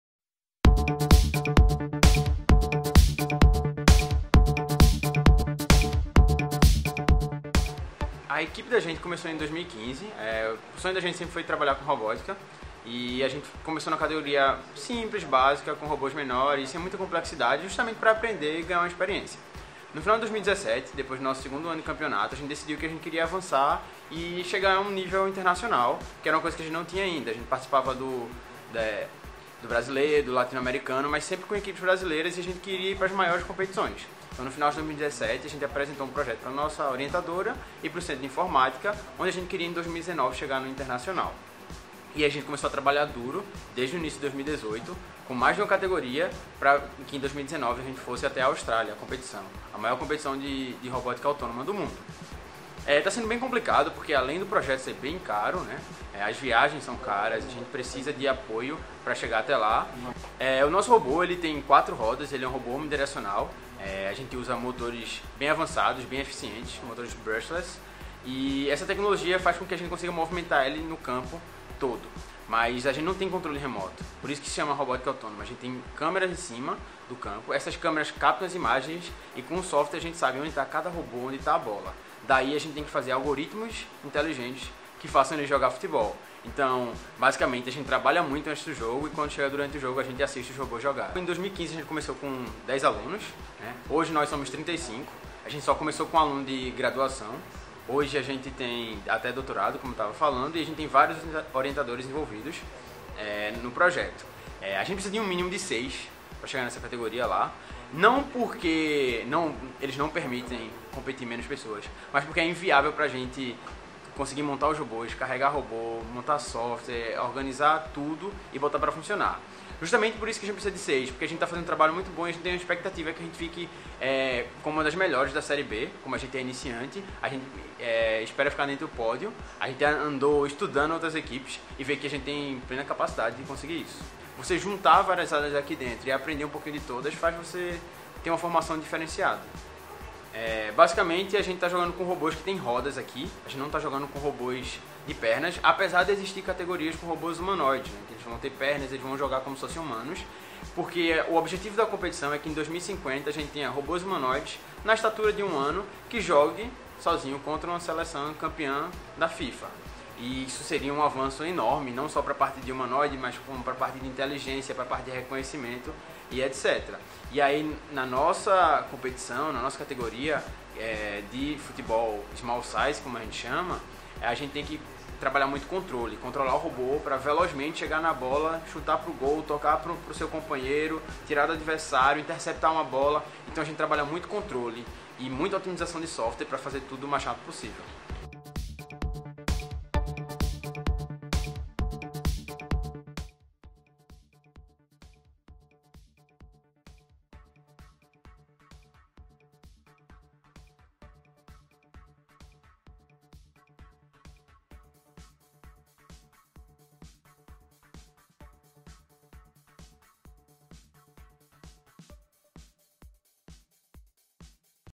LeiaJa.com - Alunos do RoboCin fazem vaquinha para ir à Austrália
Dezesseis mil quilômetros. Essa é a distância aproximada que separa 12 jovens, alunos do Centro de Informática (CIn) da UFPE, de realizar o sonho de serem campeões na maior competição de robôs autônomos do mundo, a RoboCup. Apesar de serem da única equipe do Nordeste classificada para o mundial de robótica, estudantes podem não conseguir realizar o sonho por falta de dinheiro.